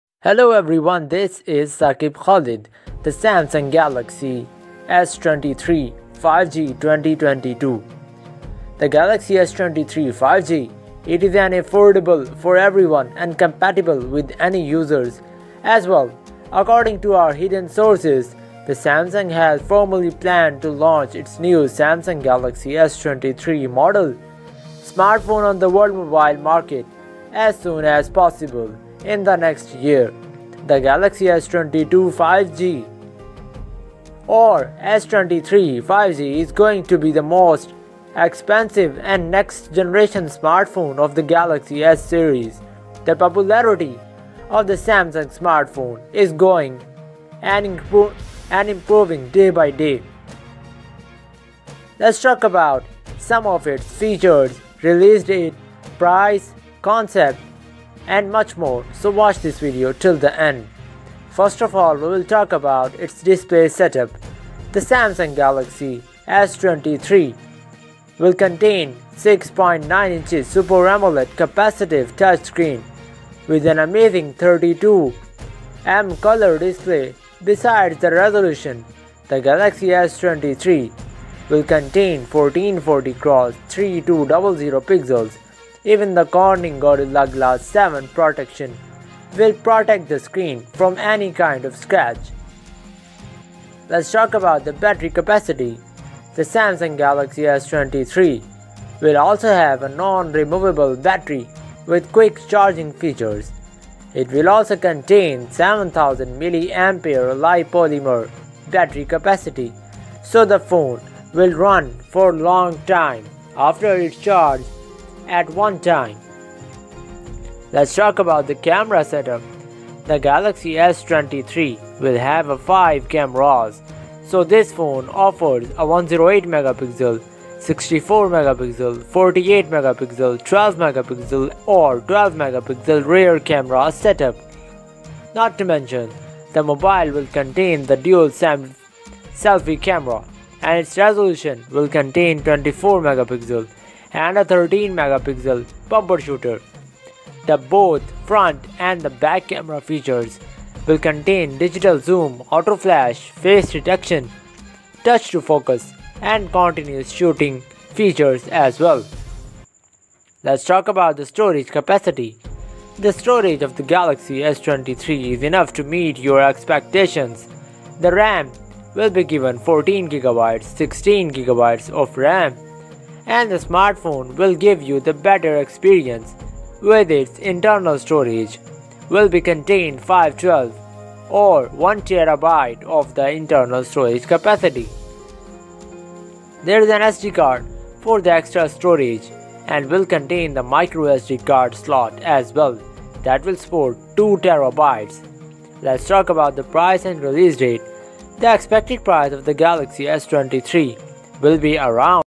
Samsung Galaxy S23 5G (2022) - Design, Concept, Price, Release Date, Specifications!!!
In this video i am going to tell you all the necessary details regarding Samsung Galaxy S23 5G (2022). I will tell you its price, release date, features, leaks, rumours, cost and many more so watch this video till the end...!
Previous Videos.